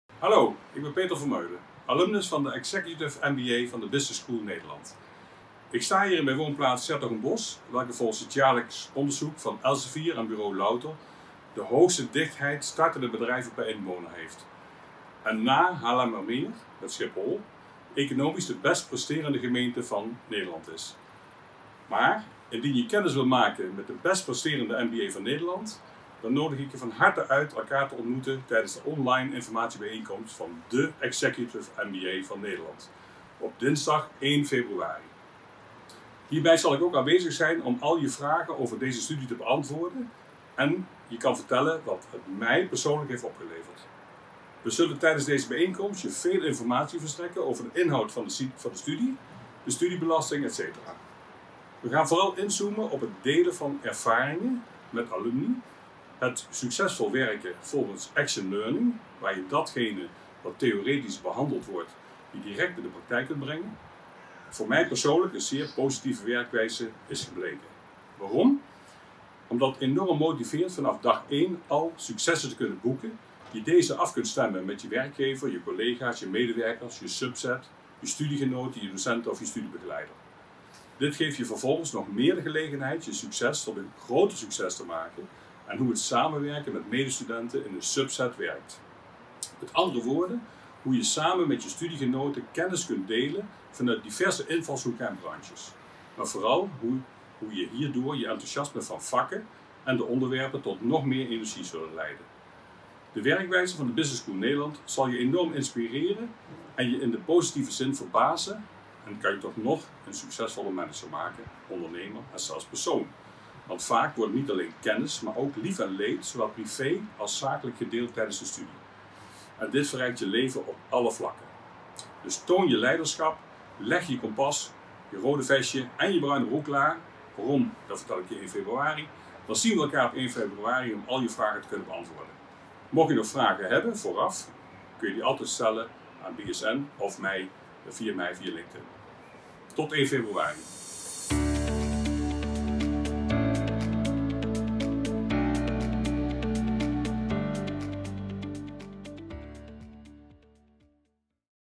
Ga in gesprek met Peter Vermeulen MBA tijdens de online informatiebijeenkomst op 1 februari!
Wat kan onze Action Learning MBA-opleiding jou brengen?

Vraag het aan onze oud-student Peter Vermeulen MBA tijdens de online informatiebijeenkomst op 1 februari (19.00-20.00 uur). Hij vertelt je onder andere graag wat de MBA hem persoonlijk heeft gebracht. Ook deelt hij zijn ervaringen met de Action Learning leermethode, de rode draad door onze MBA-opleidingen, die voor hem een positieve werkwijze is gebleken. Waarom? Hij vertelt jou erover in deze video.

Sluit online aan op 1 februari. Meld je aan op